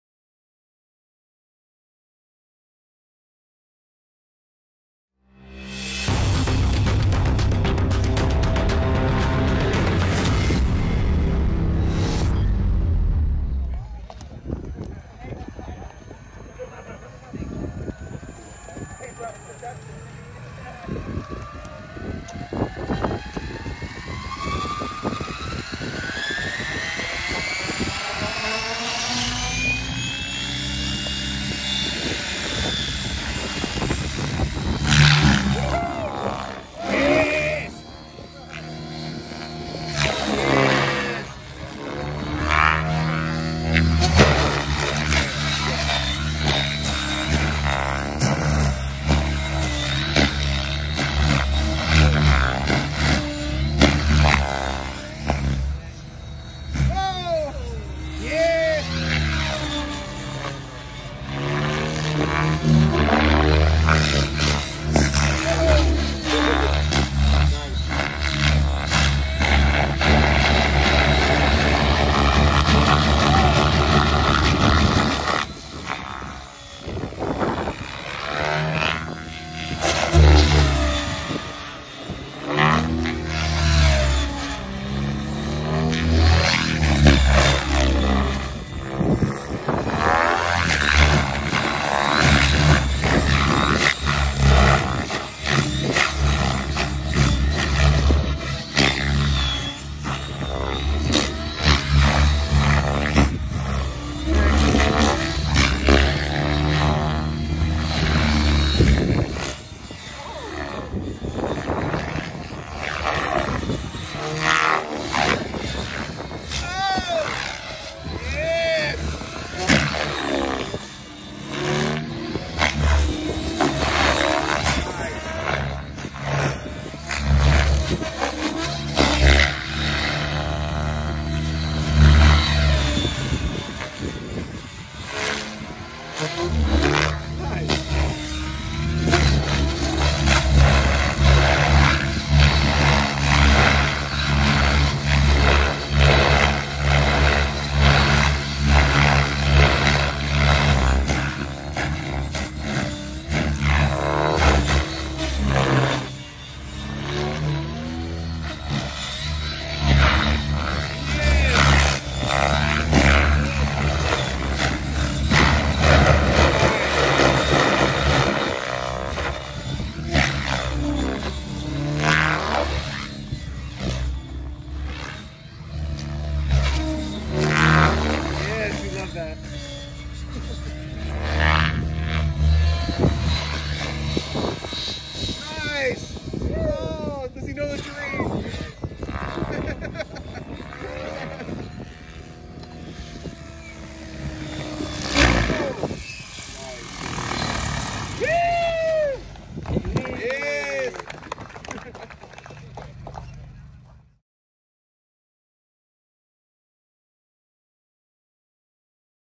Marshall Ranch Heli FunFly Noon Demo BEN STORICK
Had an awesome time out in Livermore California last weekend at the 2020 Marshall Ranch Heli FunFly, what an amazing event! Thank You Ken Marshall for all your hard work and wonderful hospitality!

Box stock Align T-Rex700X
HobbyWing 200amp
Futaba 18SZ/CGY760R Gyro
Align 5200mah 60C Battery Packs
2250 RPM Headspeed